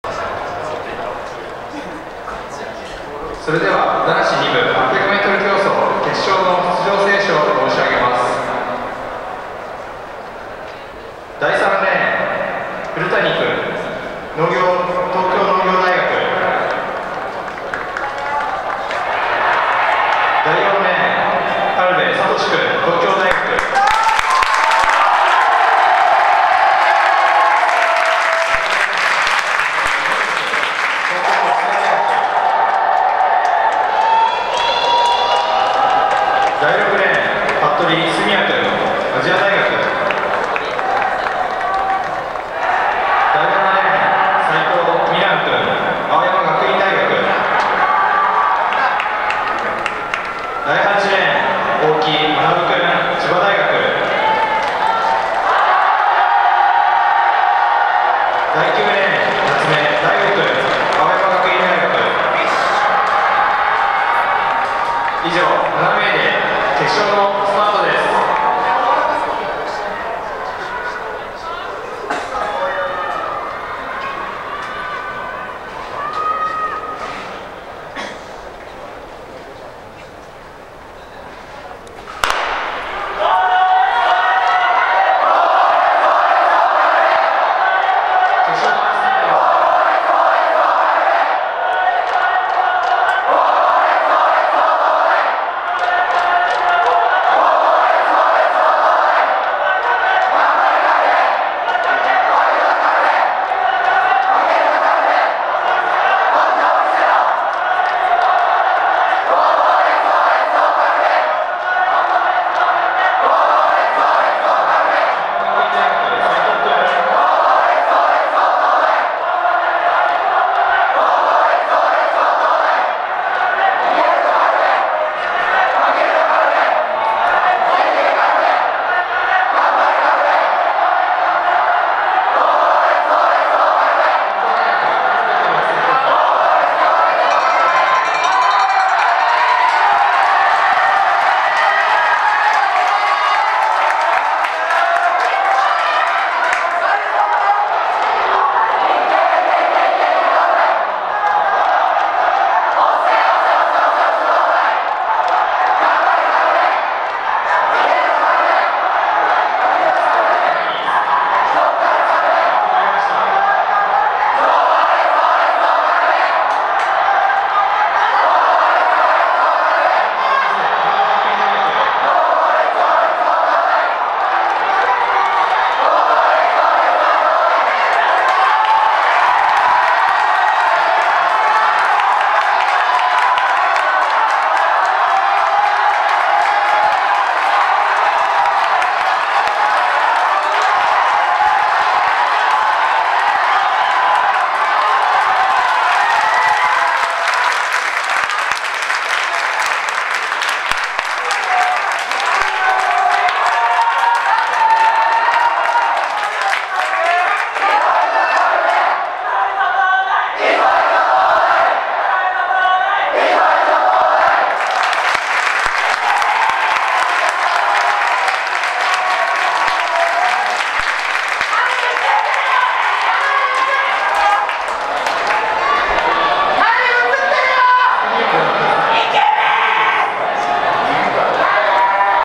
800m 男子2部 決勝 関東ｲﾝｶﾚ陸上 日産ｽﾀｼﾞｱﾑ横浜 2015.5.17
1 6 268 服部 純哉(2) ﾊｯﾄﾘ ｽﾐﾔ 亜大　埼　玉 1:51.91
2 4 486 軽部 智(3) ｶﾙﾍﾞ ｻﾄｼ 東大　東　京 1:52.78
3 5 455 鈴木 竜二(3) ｽｽﾞｷ ﾘｭｳｼﾞ 東国大　埼　玉 1:52.85
4 7 117 齋藤 未藍(2) ｻｲﾄｳ ﾐﾗﾝ 青学大　神奈川 1:53.53
5 8 579 大木 学(3) ｵｵｷ ﾏﾅﾌﾞ 千葉大　茨　城 1:54.99
6 9 118 棗 大吾(2) ﾅﾂﾒ ﾀﾞｲｺﾞ 青学大　北海道 1:55.37
7 3 646 古谷 樹仁(2) ﾌﾙﾀﾆ ﾐｷﾋﾄ 東農大　長　野 1:59.96